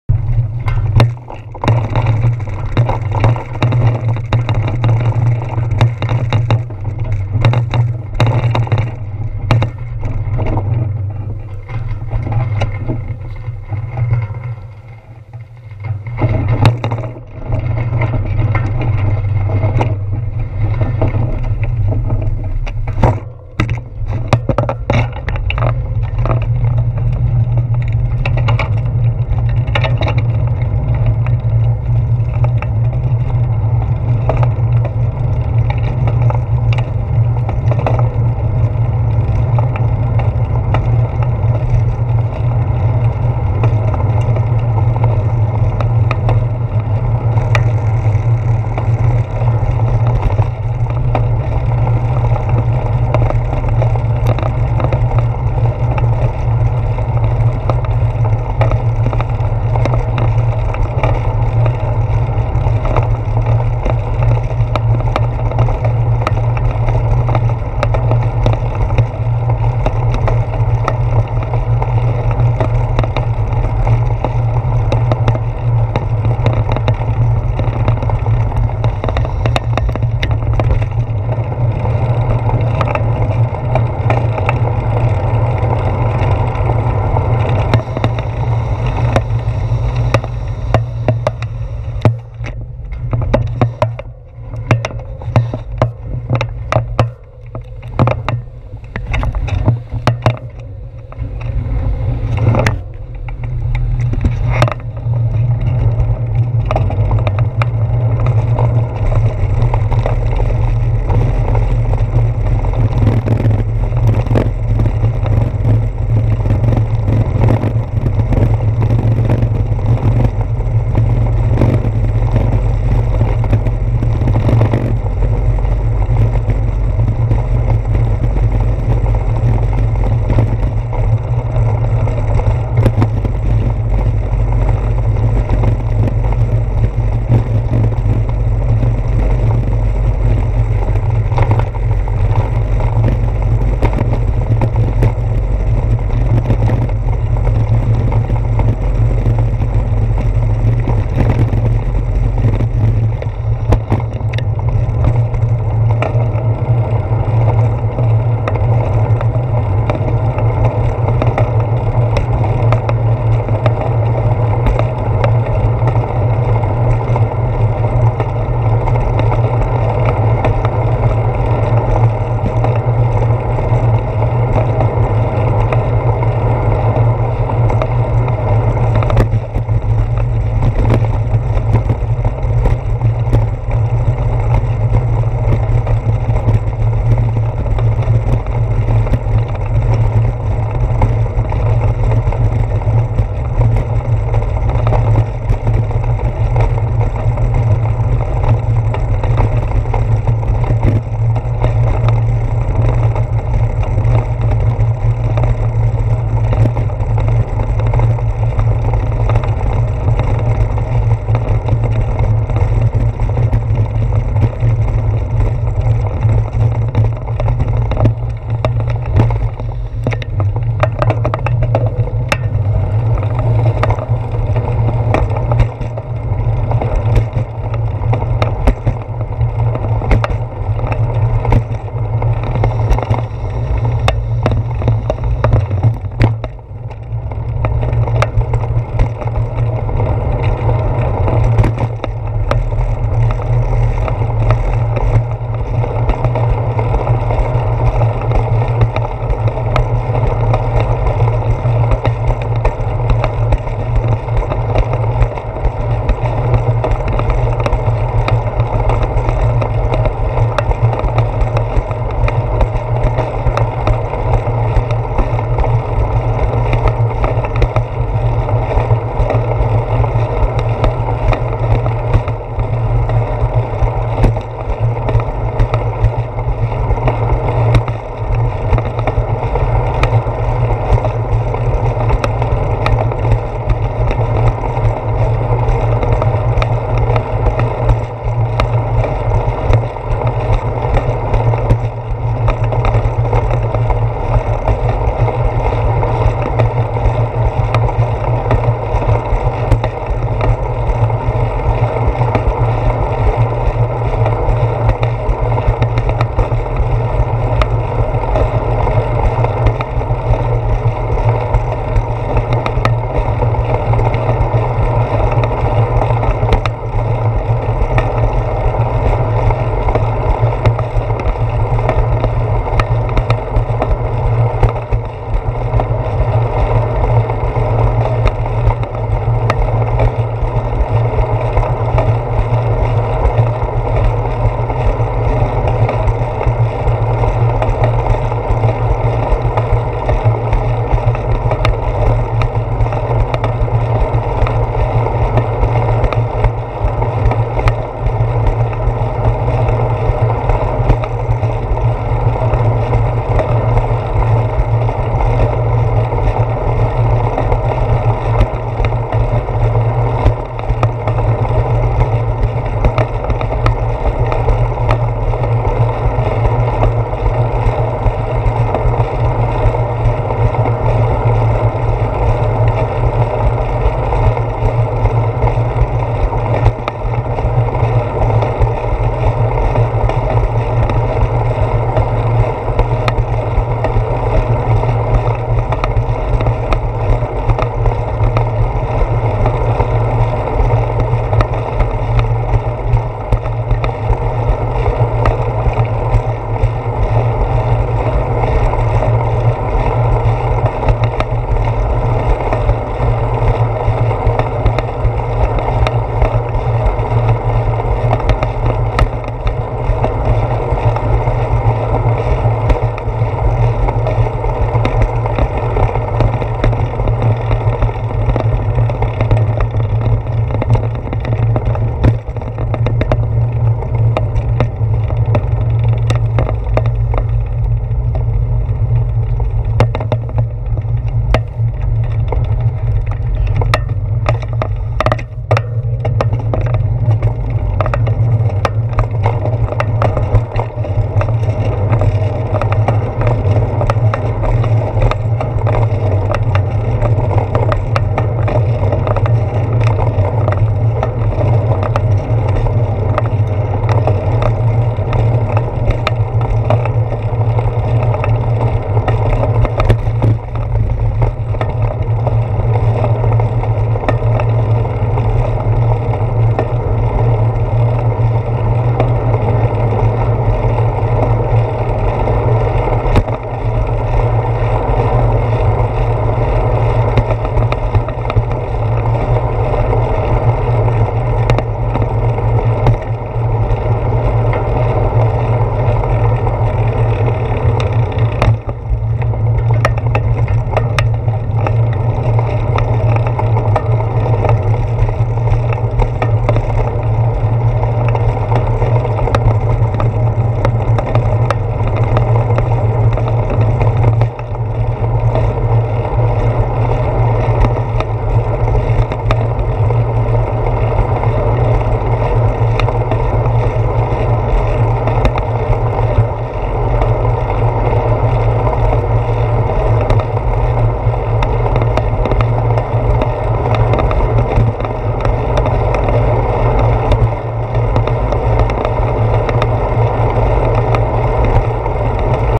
Florida Bike commute Wedneasday morning - Quiet & Humid - front camera, part 2/3
A quiet and humid morning.   There was little to no breeze in this part of Florida, save what I created by biking through the streets of Safety Harbor and Clearwater, Florida on the way to work on the morning of August 26, 2015.